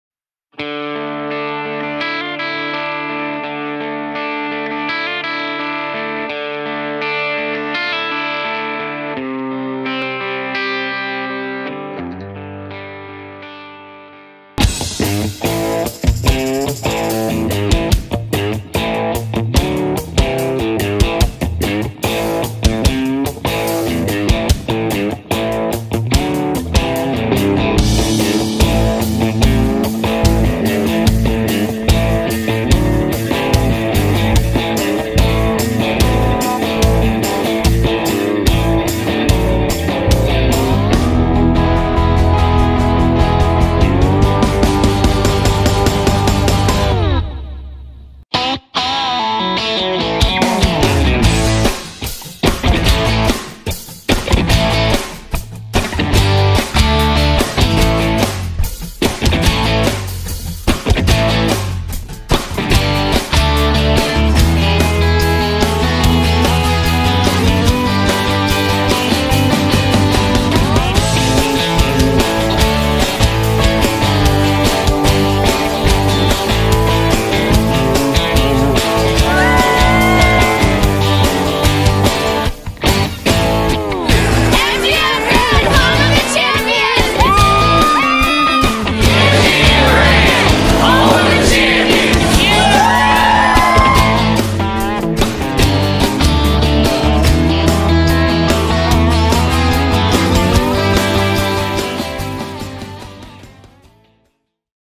NFR Highlights at the MGM Grand Las Vegas 2012
In Case You Missed It: It was an AMAZING weekend as we wrapped up a great year of NFR. With autograph sessions with Champion cowboys and FREE concerts by artists such as Lee Brice nightly at the Gold Buckle Zone, this was the place to be during the rodeo. See you in 2013!